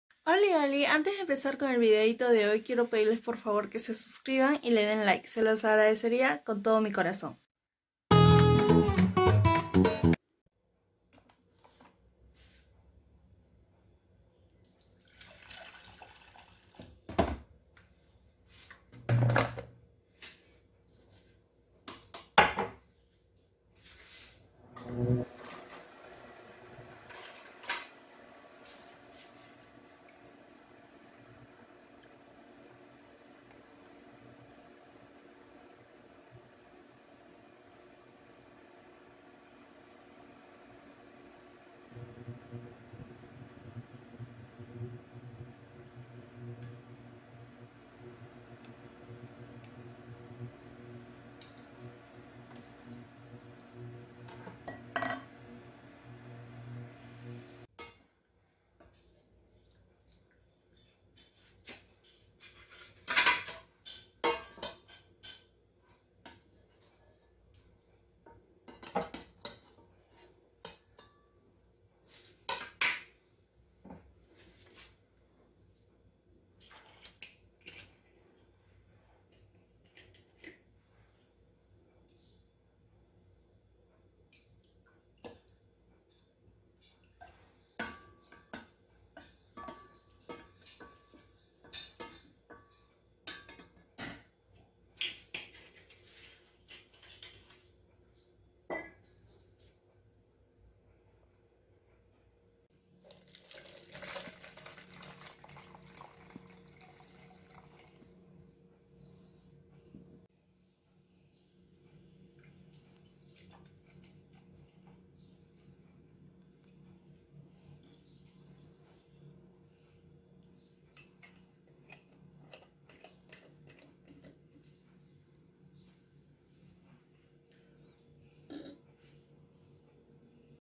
TÓNICO DESPIGMENTANTE (2)
Holi Holi...

El día de hoy estaré haciendo un tónico despigmentante...

RECUERDEN CONSULTAR CON SU DERMATÓLOGO ANTES DE ECHARSE UN TÓNICO, CABE ACLARAR QUE YO ANTES DE ECHARME EL TÓNICO EN EL ROSTRO LO CONSULTO CON MI DERMATÓLOGO